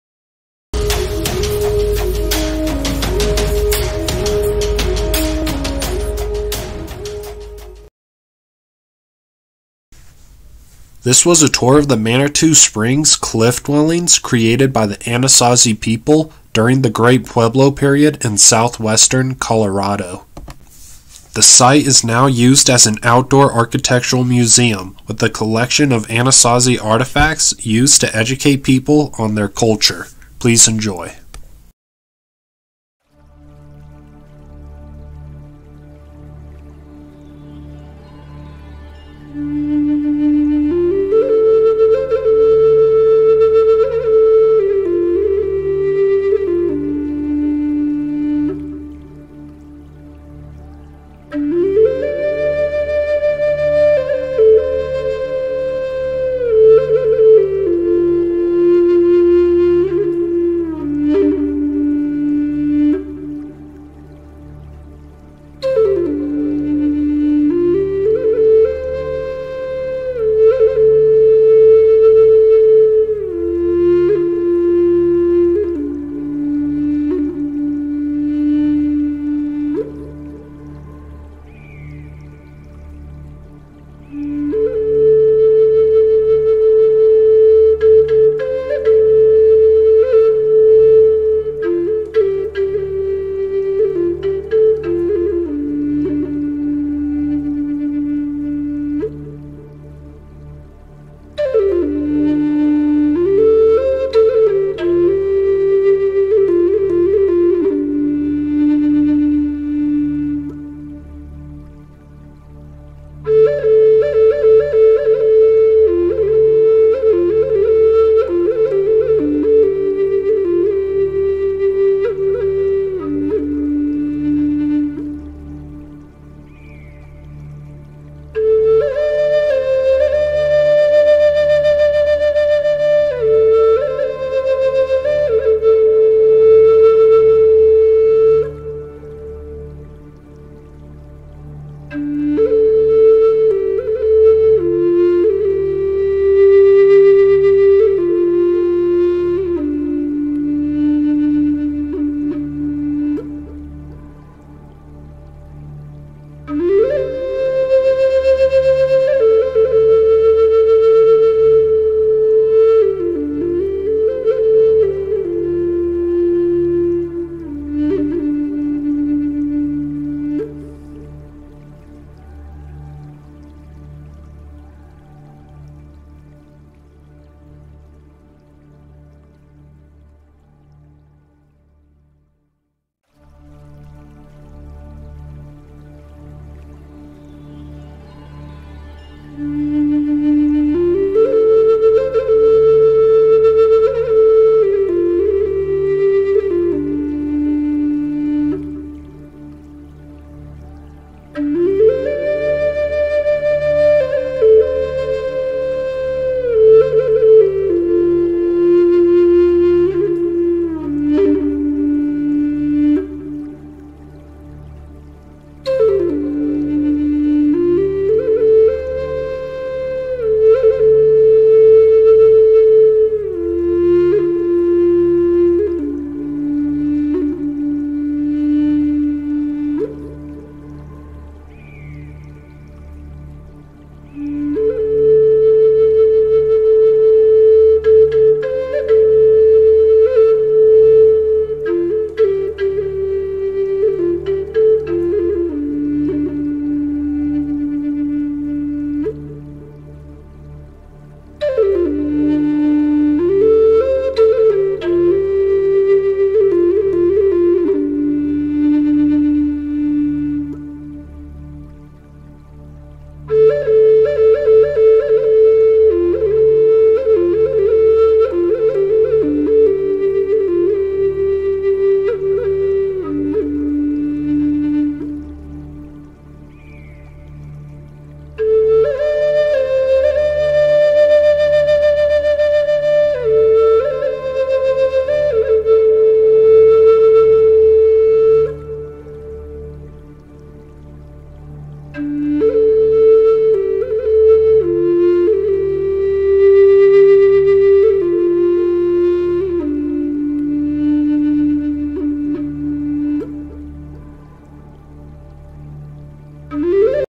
Manitou Springs Anasazi Cliff Dwelling Site Tour
I visited the Manitou Springs Anasazi Cliff Dwellings, a very interesting location and very informative. If you find yourself in Colorado and can't visit Mesa Verde I highly recommend coming here to learn about the Anasazi culture. Please enjoy!